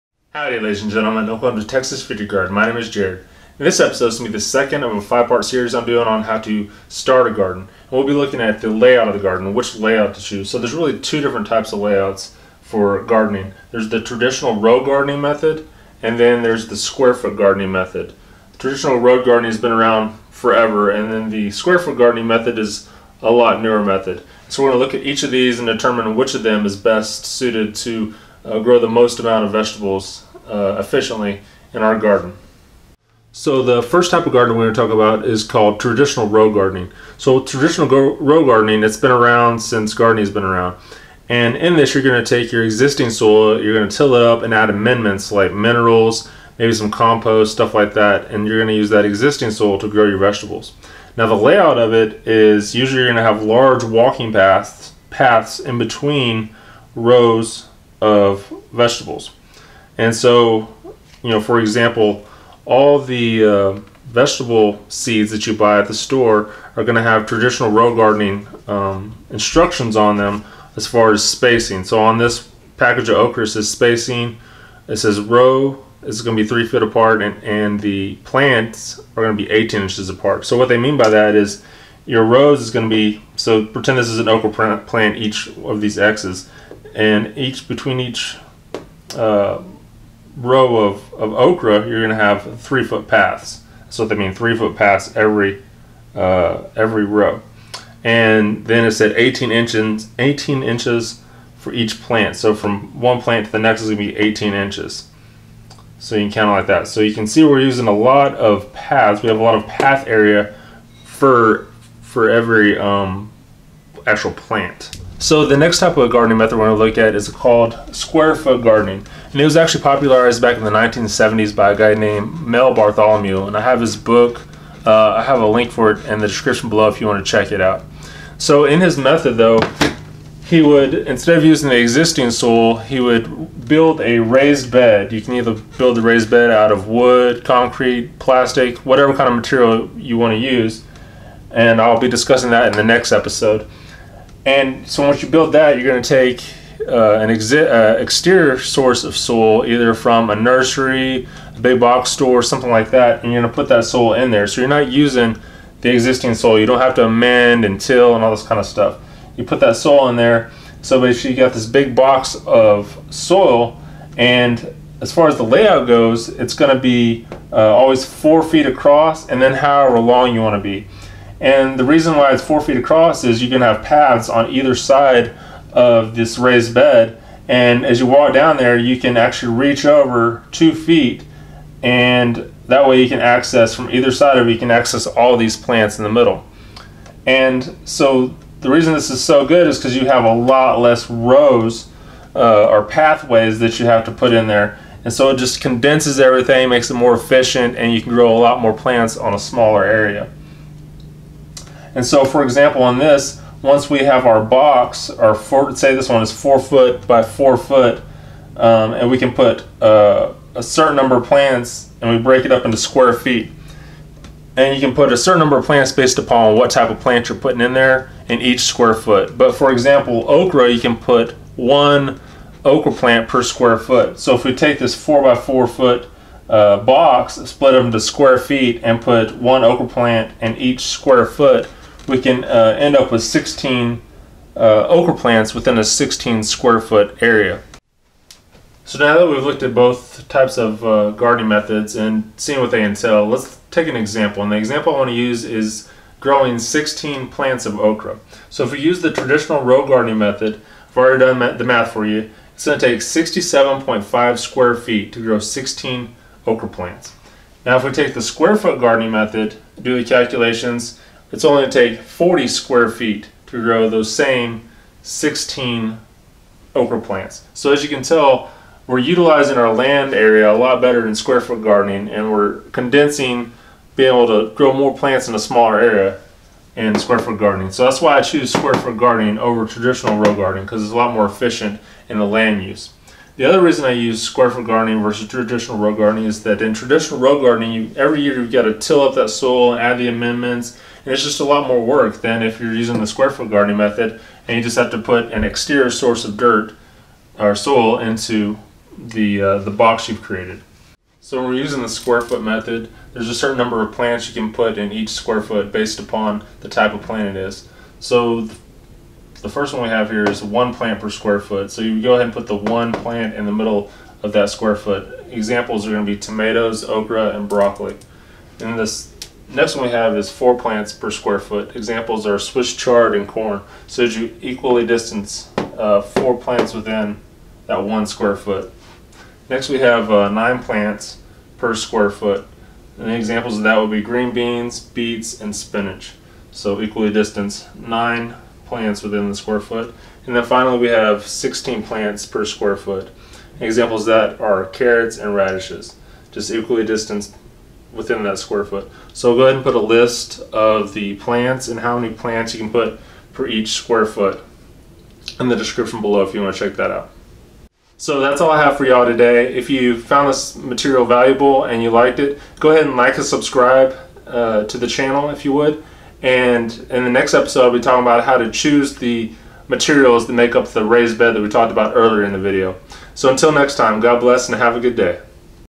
Starting a garden 2) Traditional row vs square foot gardening
This is the second video in a five part series that looks at how to start a garden.  The second consideration to make when starting a garden is determining the garden layout.  The two main types of layouts for gardening are traditional row gardening and square foot gardening.

Square foot garden plants per square foot.  These numbers come from "All New Square Foot Gardening" by Mel Bartholomew

1 plant- broccoli, cabbage, cauliflower, eggplant, okra, pepper, tomato

4 plants- Swiss chard, corn, lettuce, parsley, potato, strawberry

9 plants- green beans, beets, snap peas, spinach,

16 plants- carrot, onion, radish

1 per 2 square feet- melon, summer squash, winter squash

2 plants- cucumber (I disagree with the book on this.  I usually plant 4 to 9 per square foot and use a trellis.)

"All New Square Foot Gardening" by Mel Bartholomew